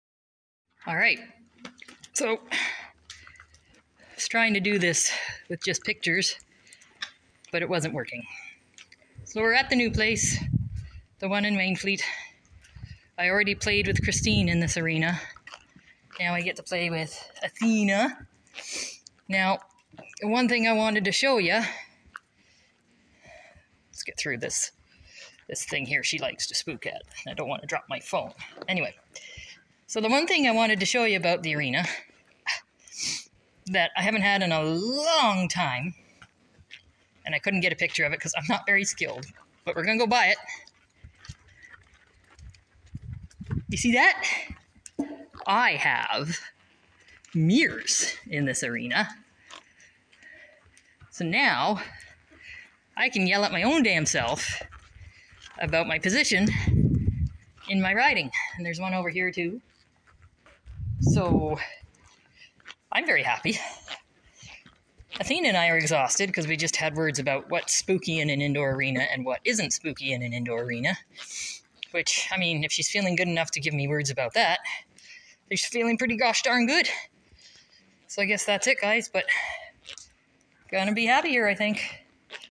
Athena`s First Time in the Indoor arena at the new place .
This one is also late guys as i was unable to upload due to provider issues .. better late then never .